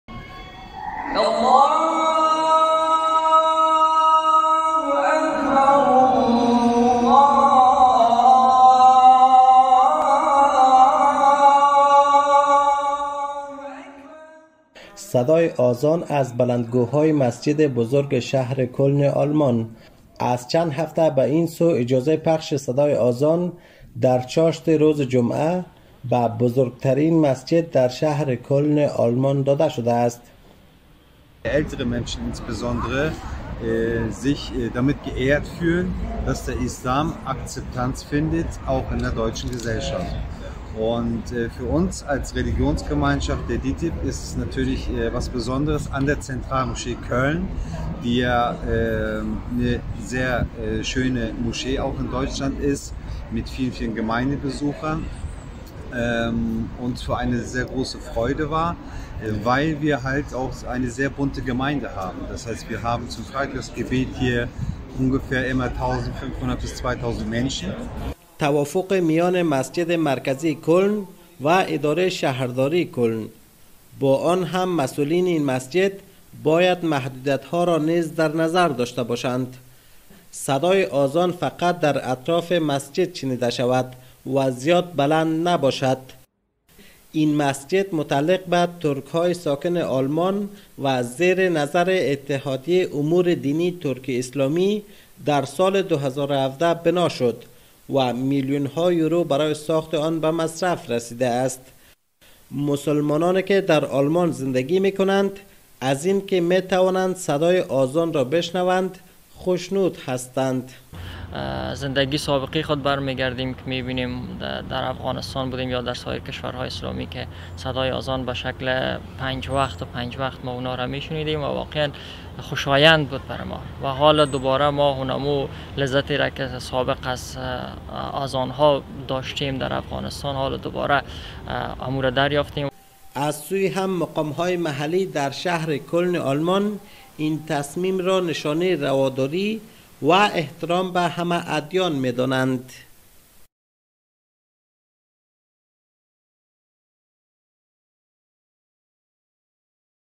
Gebetsruf In Zentraloschee Köln Neuابراز خرسندی شماری از مسلمانان از پخش اذان در شهر کلن آلمان
پس از آنکه مسجد مرکزی کلن مجوز پخش اذان نماز جمعه را دریافت کرد، حالا صدها تن از مسلمانان ساکن آلمان برای شنیدن اذان و ادای نماز به این مسجد می روند.
جزئیات بیشتر را دراین ویدیو ببینید